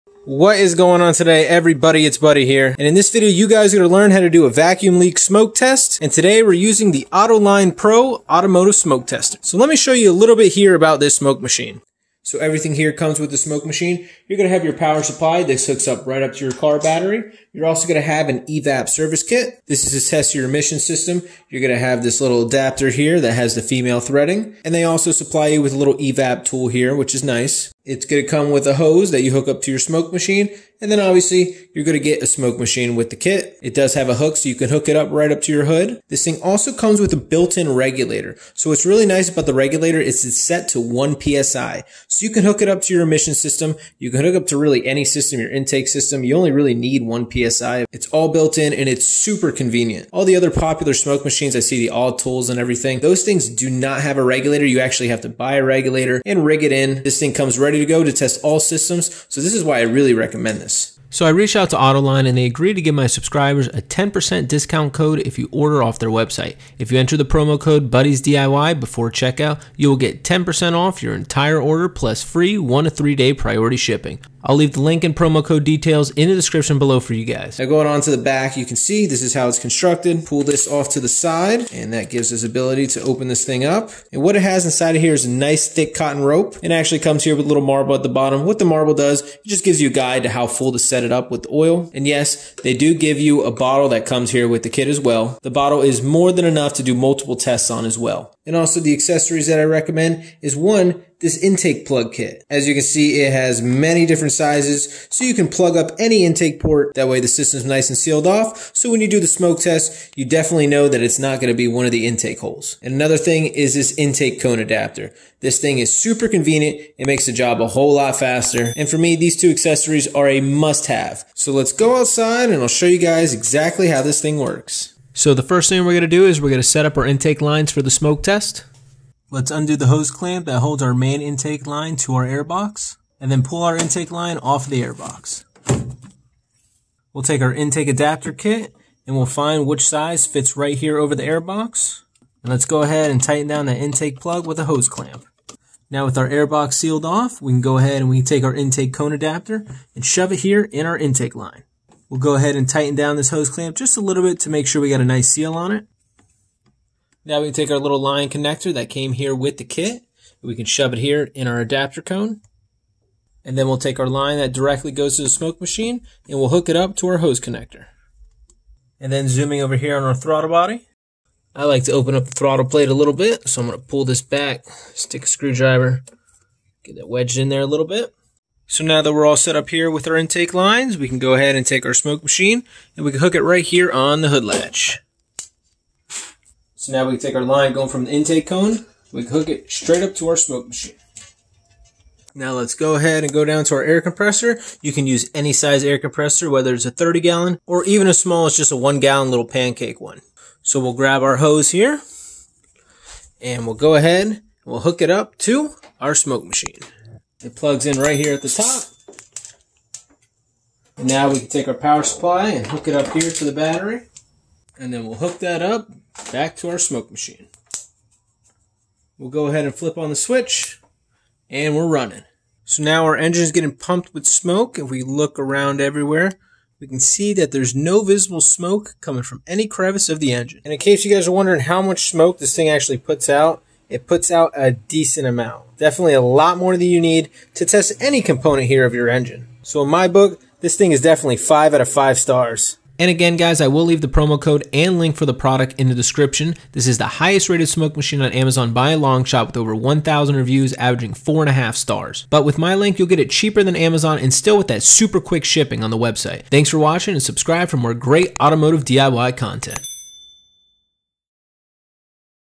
How To Test For Vacuum or EVAP Leaks | Autoline Pro Smoke Machine
Works With OR Without caps

In this video I go over how to fine a vacuum leak or an evap leak using an automotive smoke machine. the machine i will be using is the autoline pro shop series smoke machine. It has a built in regulator so you can smoke test your evap system without damaging it. smoke machines for cars can be used to find leaks in the power steering system, a/c system leak, evap system leak and vacuum leaks. Doing a smoke test will help you pass emission inspection and help you pin point where your evap leak is. A evap leak detector like this autoline pro smoke machine is going to make you life much easier. Vacuum leaks can cause rough idles, the car not to start, and many other issues wile failing emissions inspections can be costly and had to pin point a evap leak.